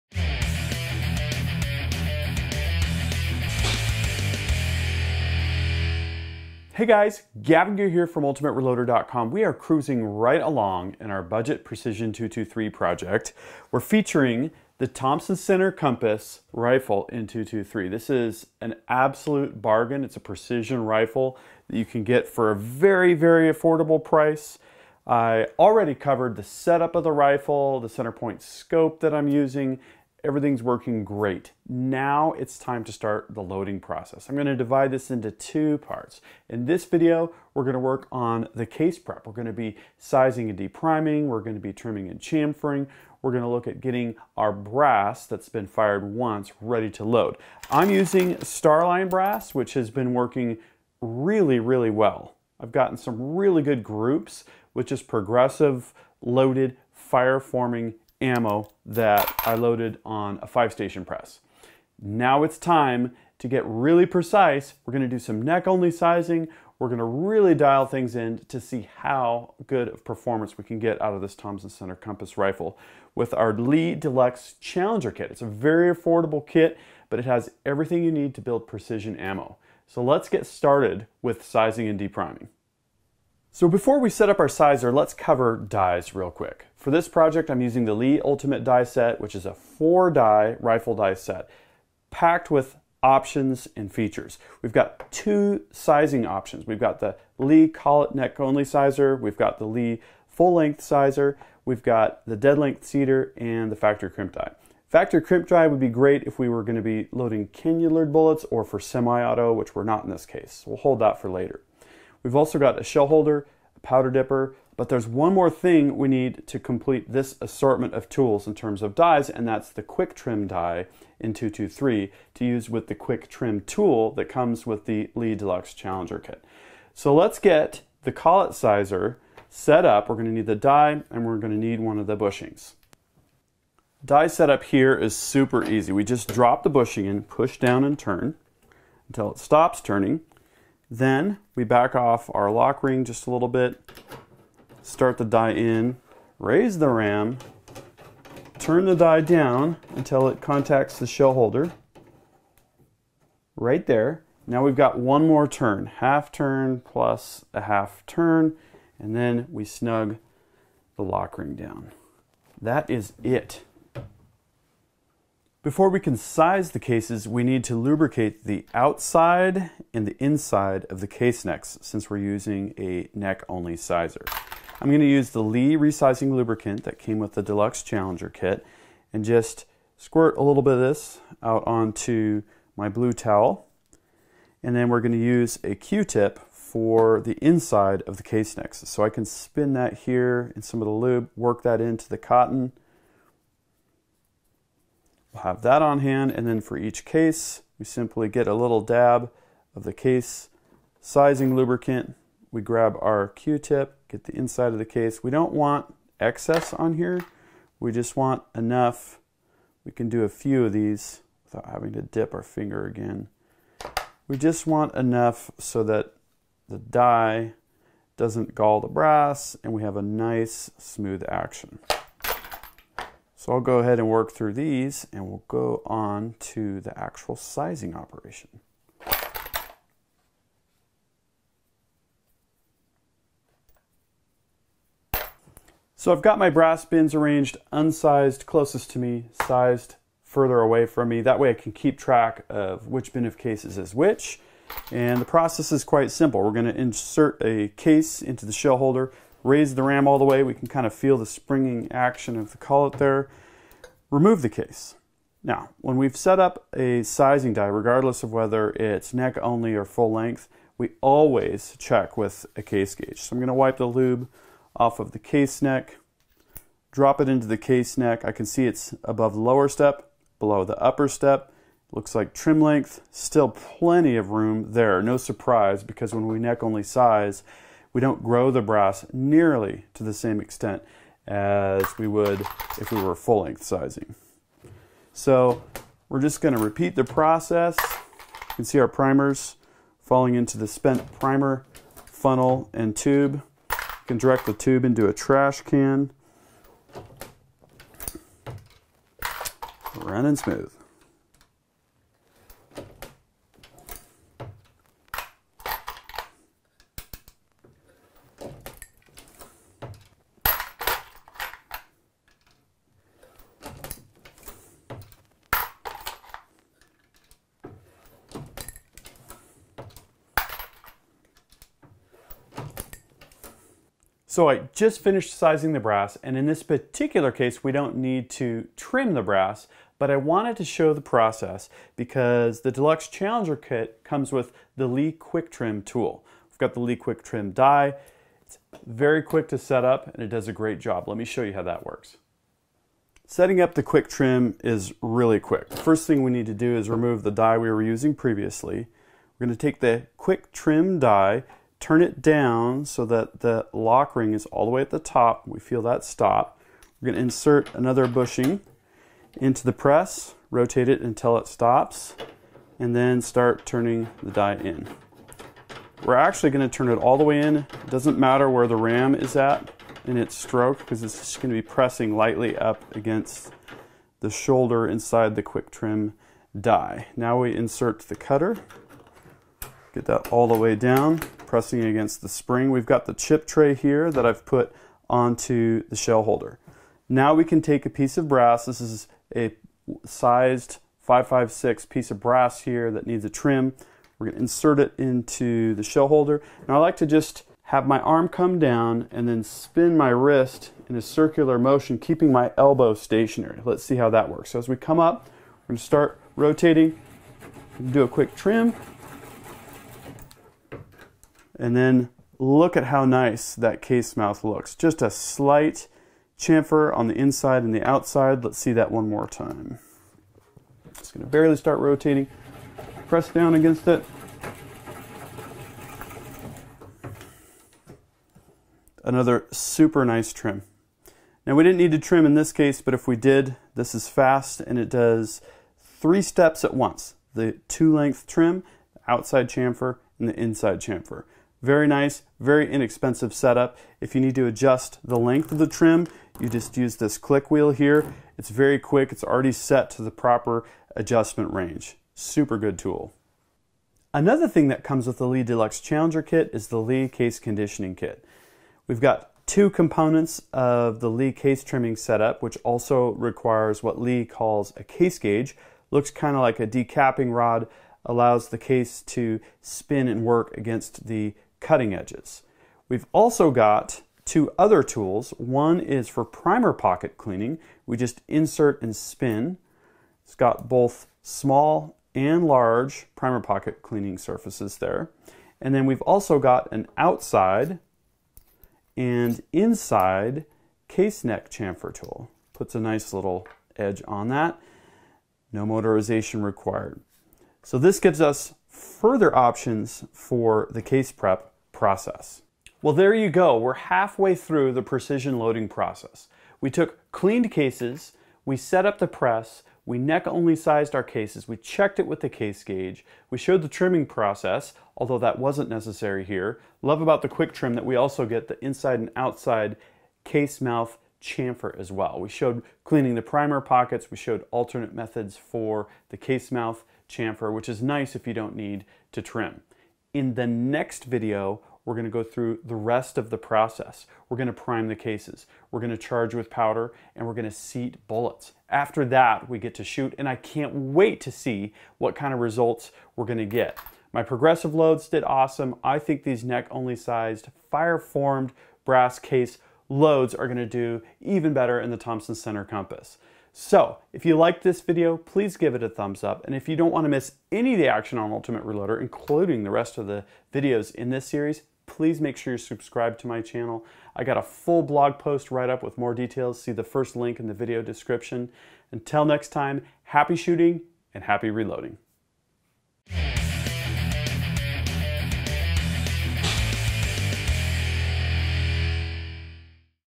Budget Precision .223: Sizing and Case Prep
It's time to start the precision reloading process with the LEE Deluxe Challenger Kit! In this video I show neck-only sizing using the LEE Collet-Type Neck Sizer, inspecting the case with a case gage, trimming the brass (with chamfer), and cleaning primer pockets. Next up: priming, charging, and seating!

*Note: case lube is optional when using the LEE Collet neck sizer.

Ultimate Reloader LLC / gavintoobe Material Connection Notice:

The following Ultimate Reloader partners are featured in this video:
- LEE Precision